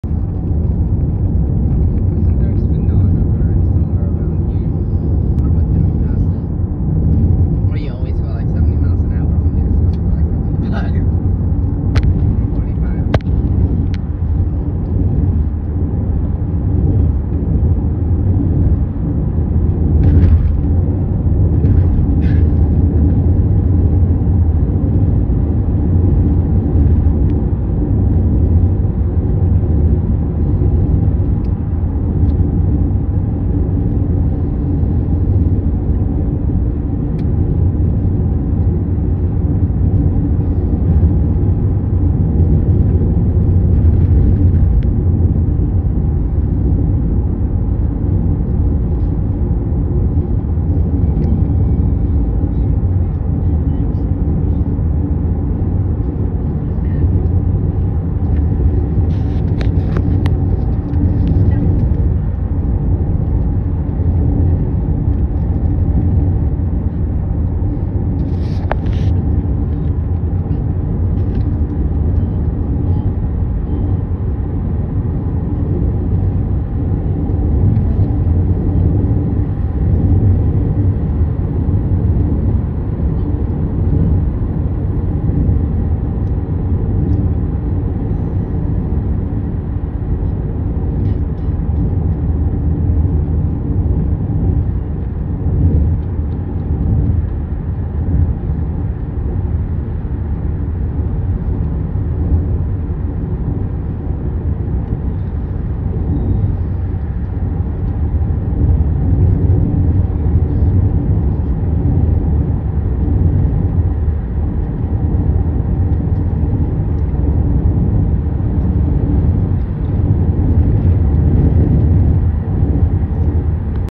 Ganda ng view
Rainer Oregon and Longview bridge and Columbia river .. driving